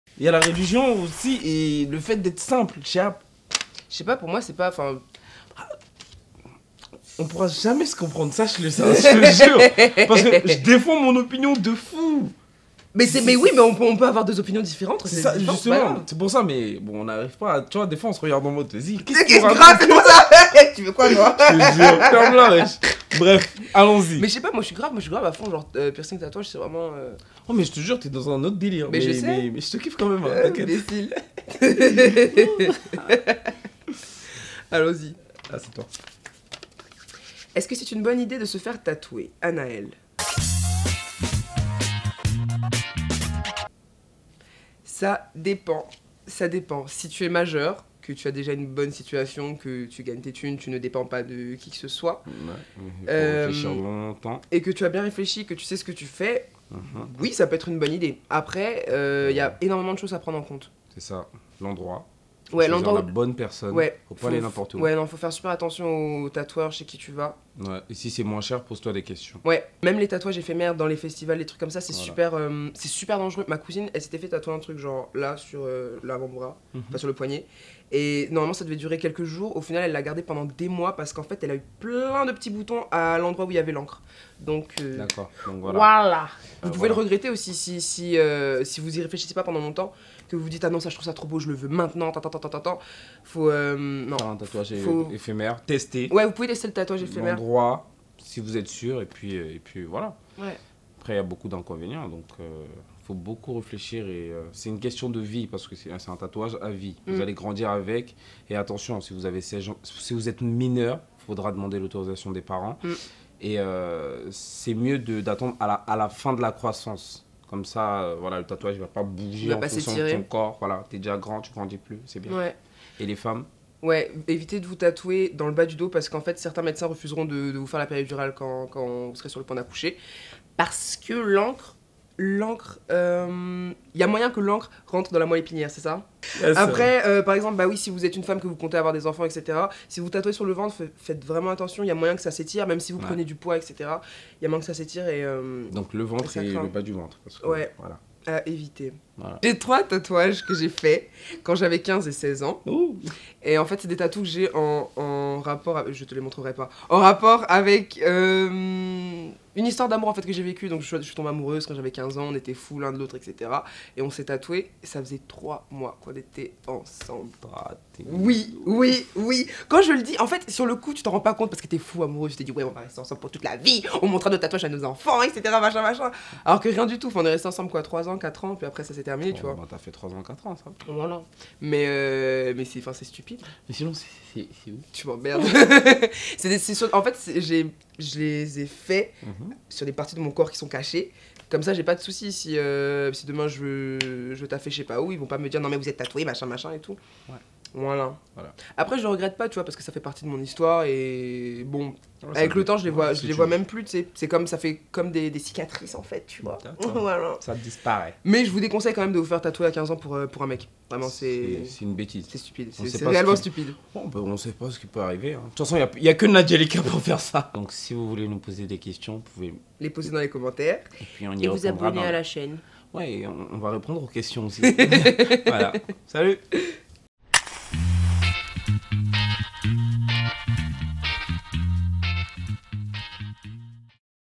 ASQ - Se faire tatouer : est-ce une bonne idée ? ft Nadjélika & Sacko
Se faire un tatouage, est-ce une bonne idée ?
 ASQ = Art x Secret x Question